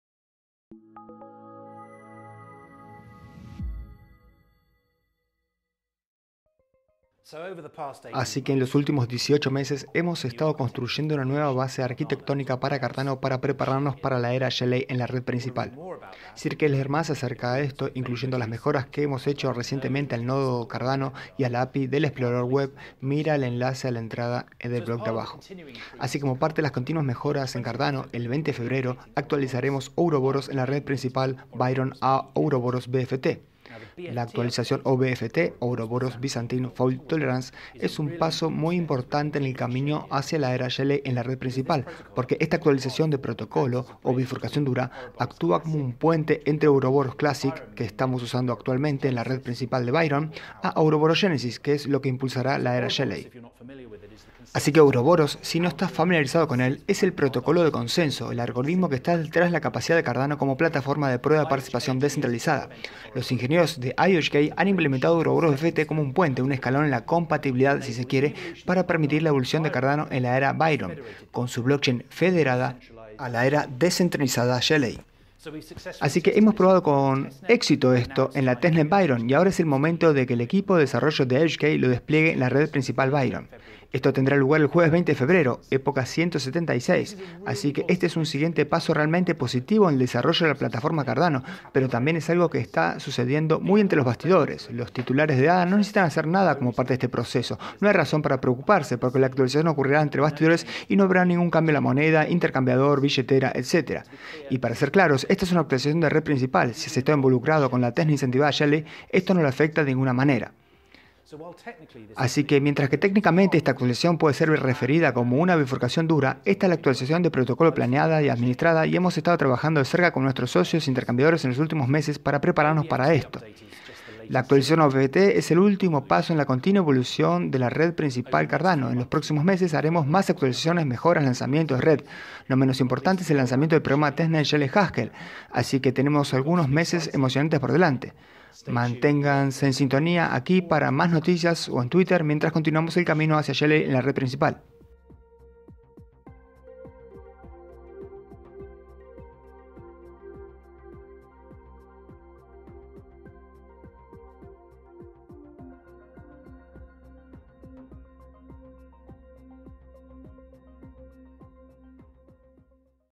Actualización Ouroboros BFT | IOHK 19 Feb 2020
Doblaje al español de "Ouroboros BFT Update"

Telegram Hispano: ADA - Cardano Español

Telegram Hispano dedicado a ejecutar nodos: Manualidades Cardano

Así que en los últimos 18 meses, hemos estado construyendo una nueva base arquitectónica para Cardano para prepararnos para la era Shelley en la red principal.

Si quieres leer más sobre eso, incluyendo las mejoras que hemos hecho recientemente al Nodo de Cardano, y a la API del explorador web, mira el enlace a la entrada del blog de abajo.

Así que como parte de las continuas mejoras en Cardano, el 20 de Febrero, actualizaremos Ouroboros en la Red Principal Byron a Ouroboros BFT.

La actualización OBFT, Ouroboros Byzantine Fault Tolerance, es un paso muy importante en el camino hacia la era Shelley en la red principal, porque esta actualización de protocolo, o bifurcación dura, actúa como un puente entre Ouroboros Classic, que estamos usando actualmente en la red principal de Byron, a Ouroboros Genesis, que es lo que impulsará la era Shelley.

Así que Ouroboros, si no estás familiarizado con él, es el protocolo de consenso, el algoritmo que está detrás de la capacidad de Cardano como plataforma de prueba participación descentralizada. Los ingenieros de IOHK han implementado Ouroboros BFT como un puente, un escalón en la compatibilidad, si se quiere, para permitir la evolución de Cardano en la era Byron, con su blockchain federada, a la era descentralizada de Shelley.

Así que hemos probado esto con éxito en la Testnet Byron, y ahora es el momento de que el equipo de desarrollo de IOHK lo despliegue en la red principal Byron.

Esto tendrá lugar el Jueves 20 de Febrero, época 176. Así que este es un siguiente paso realmente positivo en el desarrollo de la plataforma Cardano, pero también es algo que está sucediendo muy entre bastidores.

Los titulares de ADA no necesitan hacer nada como parte de este proceso.

No hay razón para preocuparse, porque la actualización ocurrirá entre bastidores, y no habrá ningún cambio en la moneda, o en la cuenta de intercambiador, en la billetera, etcétera.

Y para ser claros, esta es una actualización en la red principal. Si has estado involucrado con la Testnet Incentivada Shelley, esto no lo afecta de ninguna manera.

Así que mientras que técnicamente esta actualización puede ser referida como una bifurcación dura, esta es una actualización de protocolo planeada y administrada, y hemos estado trabajando de cerca con nuestros socios de intercambiadores en los últimos meses para prepararnos para esto y hacer que las cosas vayan sin problemas.

La actualización OBFT es sólo el último paso en la continua evolución de la Red Principal de Cardano, y en los próximos meses, haremos más actualizaciones, mejoras y lanzamientos de la red. No menos importante es el lanzamiento del programa de Testnet Shelley Haskell. Así que tenemos algunos meses emocionantes por delante. Manténgase en sintonía aquí para más noticias, o en Twitter, mientras continuamos el camino hacia Shelley en la red principal.